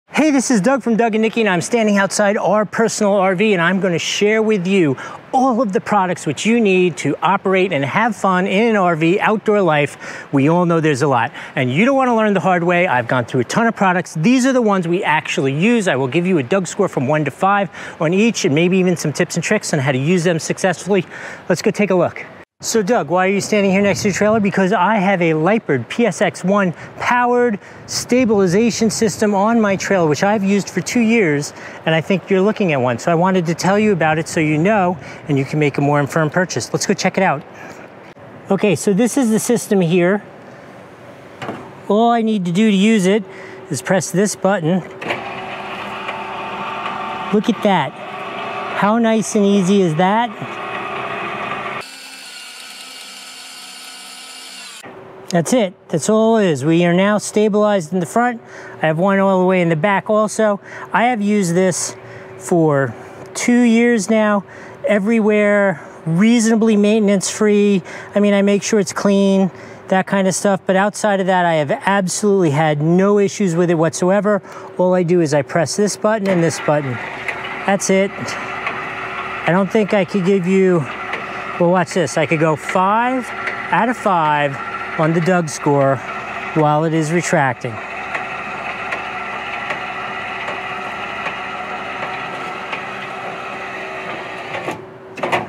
Lippert Components High Speed Power Stabilizer Jack Kit with Black Waterproof Switch Kit (298707)
Includes waterproof switch, harness and reset breaker
Legs extend up to 30”
DIMENSIONS: 79” L x 6. 2” W x 19” H, 58 pounds
POWERFULLY DESIGNED — No more cranking. With the PSX1, campers can stop wasting time and energy with manual cranks. A flip of a switch is all it takes to take the sway out of your RV or trailer.
RUGGED VERSATILITY — We designed our stabilization system to automatically adjust to uneven, rugged terrain. Each leg operates independently, navigating the surface until safely secured in place.Linc remote compatible
DURABLE CONSTRUCTION — Built with heavy gauge steel, embossed legs, and a powder-coat finish — when it comes to durability, this power stabilizer was made to withstand the elements and perform with efficiency, day after day.
THOUGHTFUL INNOVATION — We’ve equipped our PSX1 with integrated breaker technology designed to protect users from damaging their RV by tripping the breaker if the circuit is overloaded while lifting the trailer — and it helps prevent side-to-side movement while extending and retracting.